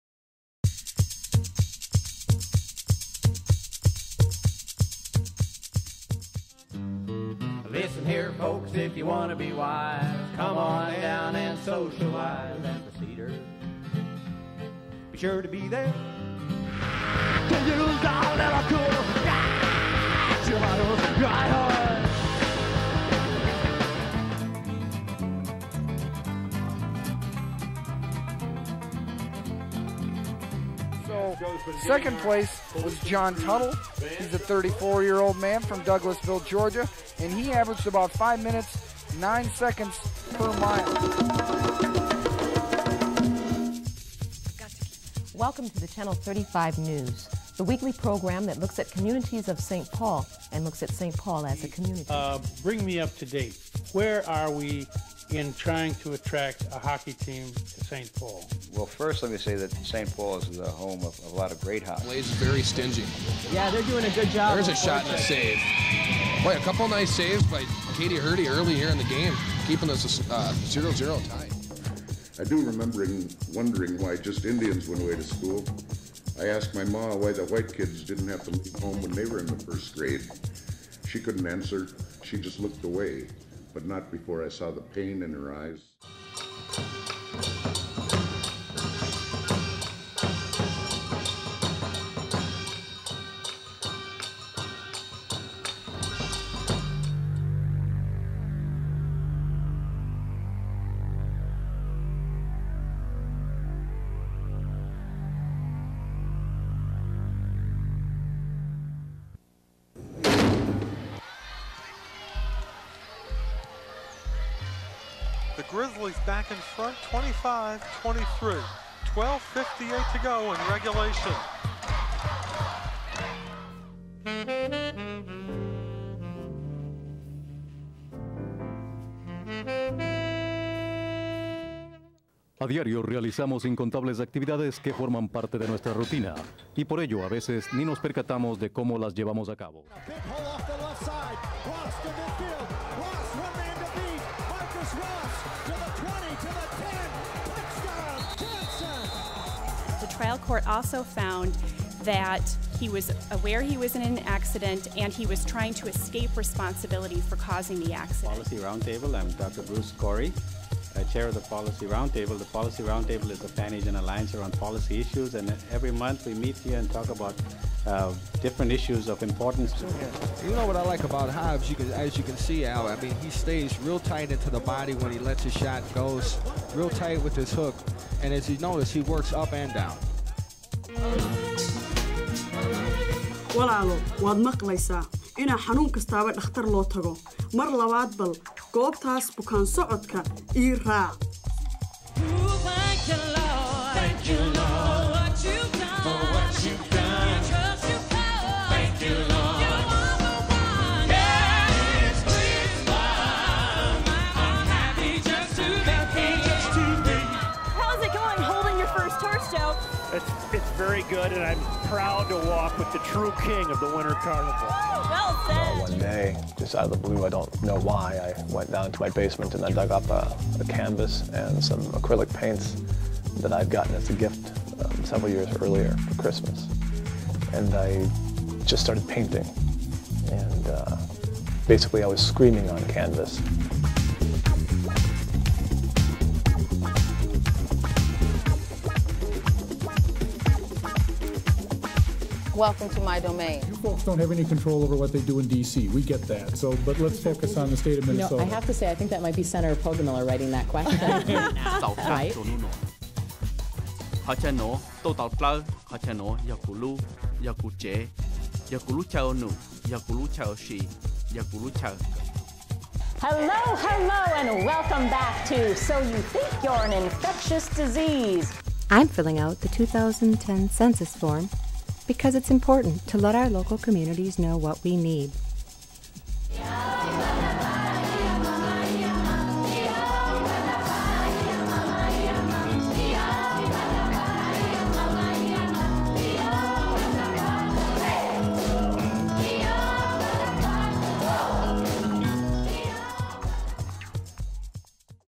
SPNN's 25th Anniversary - A Look Back
Celebrating 25 years of producing community media, here's a look back at some of the work produced by the Community Productions Department over the past two decades.